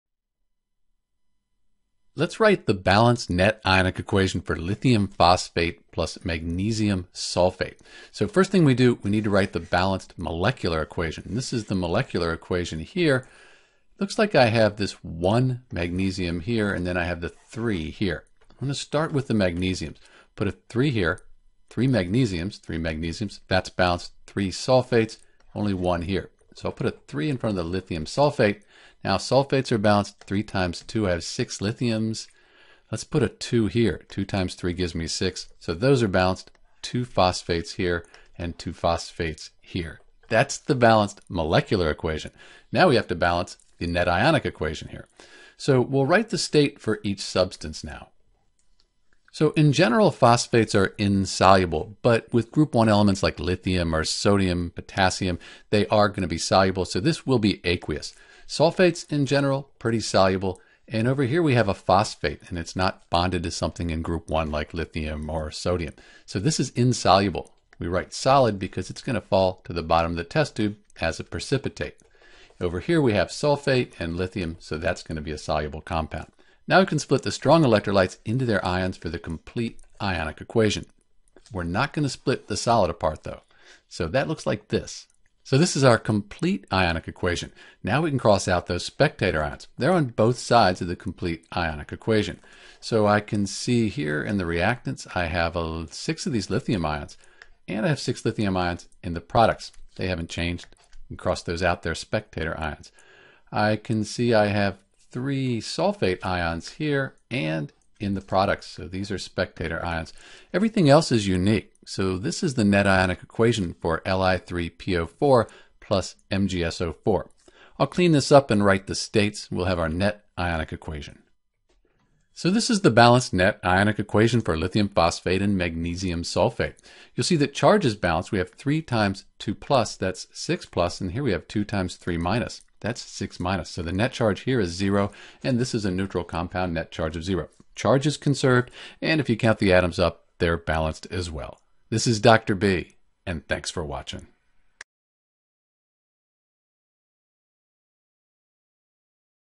How to Write the Net Ionic Equation for Li3PO4 + MgSO4 = Mg3(PO4)2 + Li2SO4
There are three main steps for writing the net ionic equation for Li3PO4 + MgSO4 = Mg3(PO4)2 + Li2SO4 (Lithium phosphate + Magnesium sulfate). First, we balance the molecular equation. Second, we write the states and break the soluble ionic compounds into their ions (these are the strong electrolytes with an (aq) after them). Finally, we cross out any spectator ions. These are the ions that appear on both sides of the ionic equation.

Important Skills

More Practice

General Steps:

1. Write the balanced molecular equation.
2. Write the state (s, l, g, aq) for each substance.
3. Split soluble compounds into ions (the complete ionic equation).
4. Cross out the spectator ions on both sides of complete ionic equation.
5. Write the remaining substances as the net ionic equation.